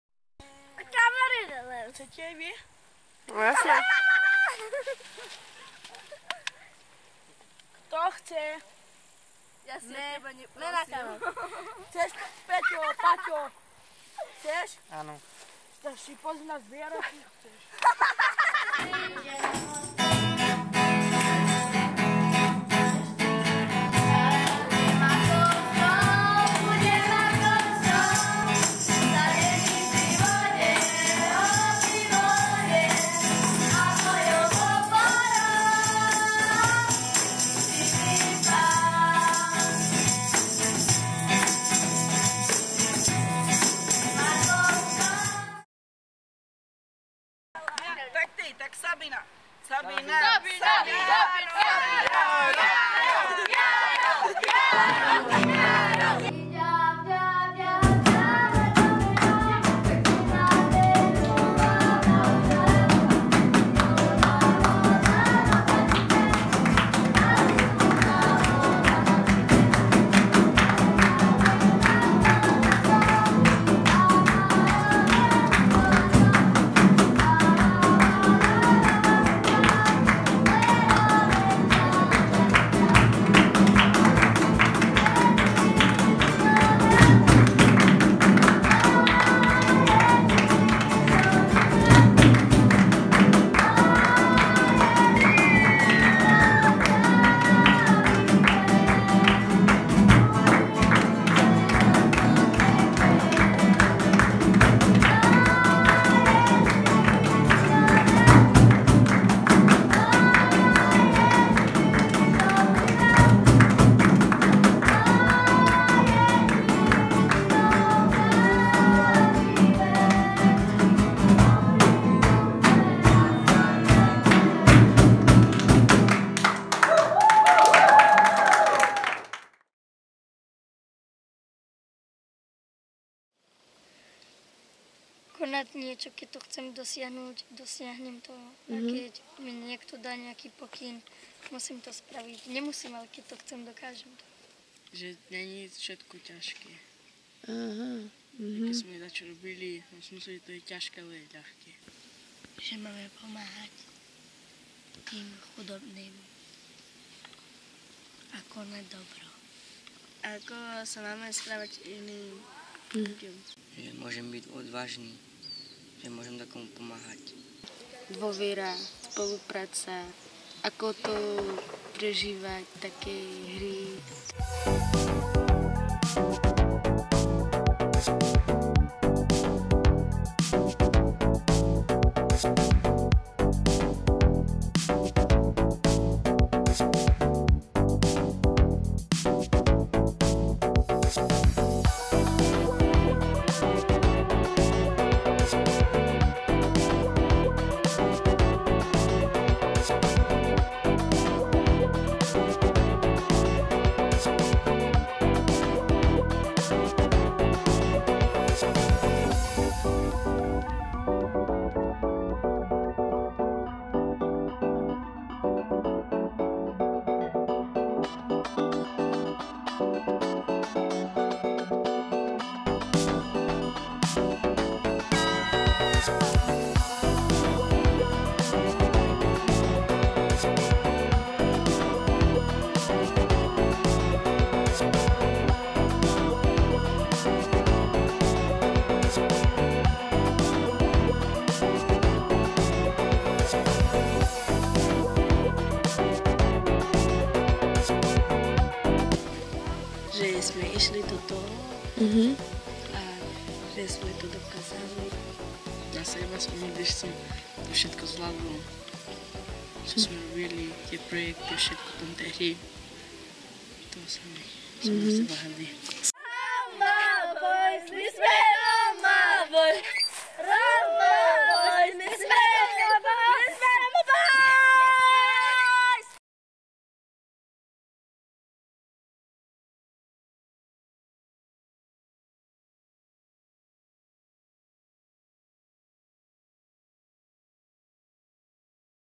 Hrdinovia 2018
Projekt Support and empower Roma youth využíva neformálne vzdelávanie a dobrovoľníctvo na posilnenie mladých ľudí zo sociálne vylúčených komunít. Projekt bol podporený programom Erasmus+ a realizovaný ako strategické partnerstvo medzi Elményakadémia, Človek v ohrození a Outward Bound Slovakia.

Video: Tatiana Černochová